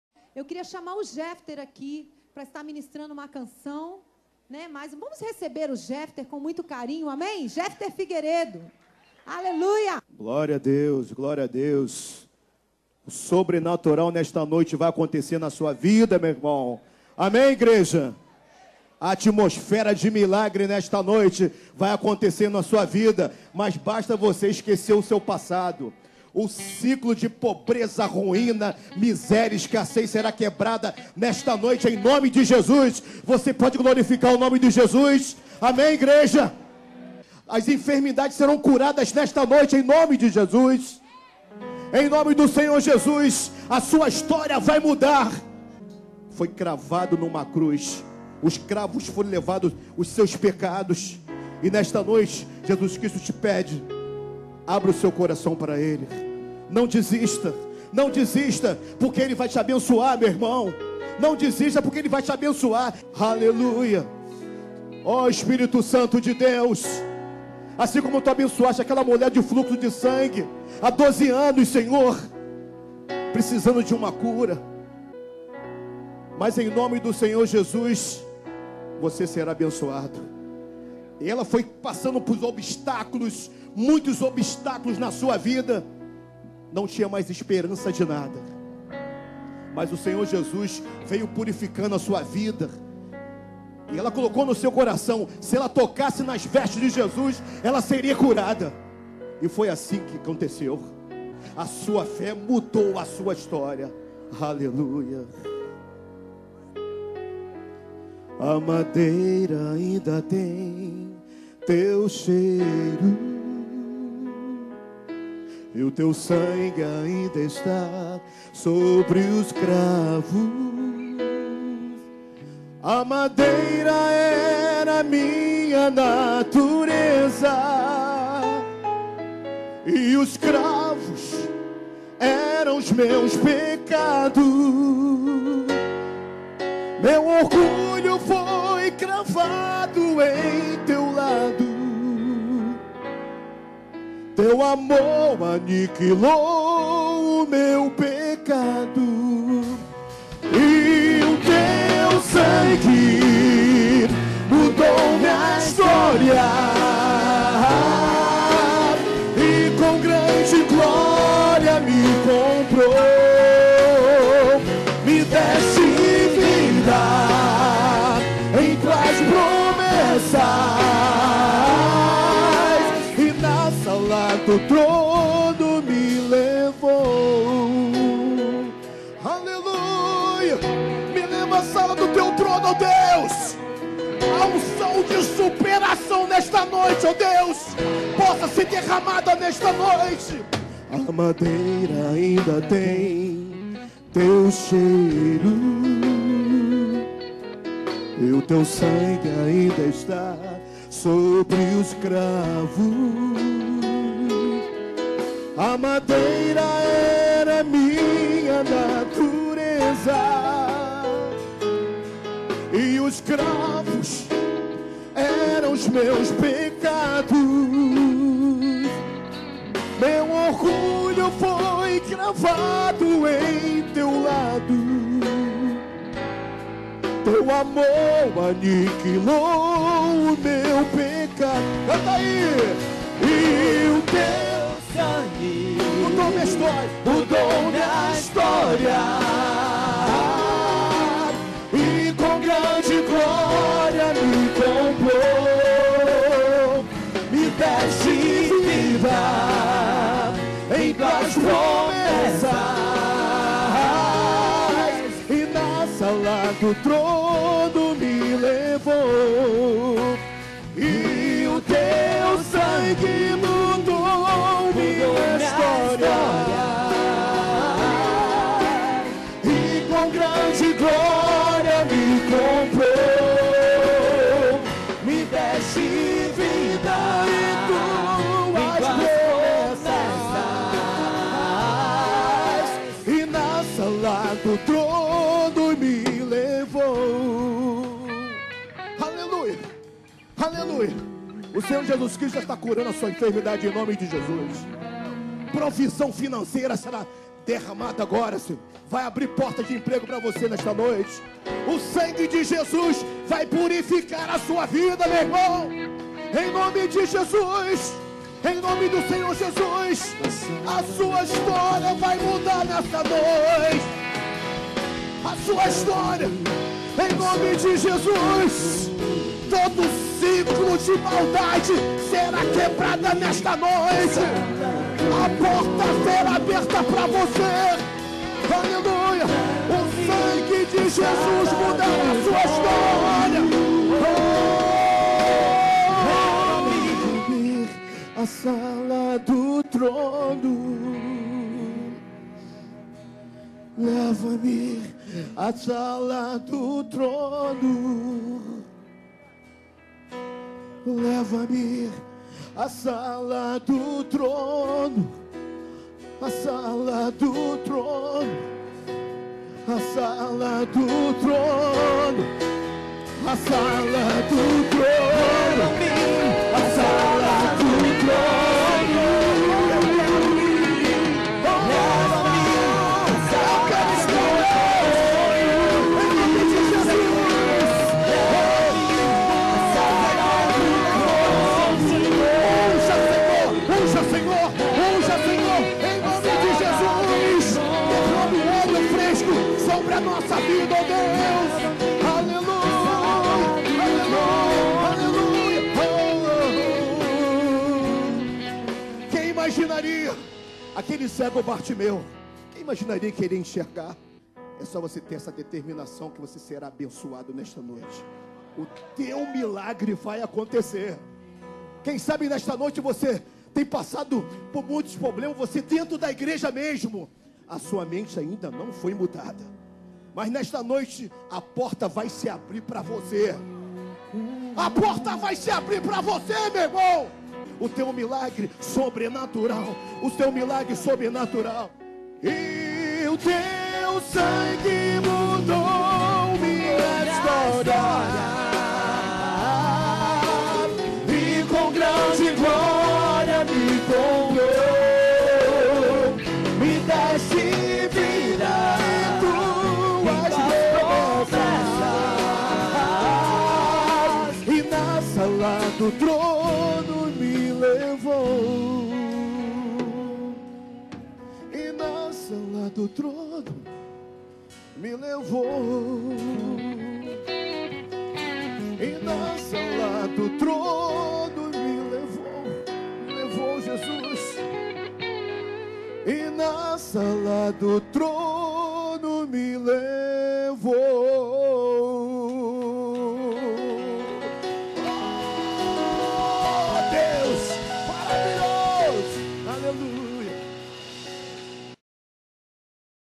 Jefter Figueiredo Ministra no Ministério Apascentar de Nova Iguaçu
Uma noite Sobrenatural , Deus operou poderosamente.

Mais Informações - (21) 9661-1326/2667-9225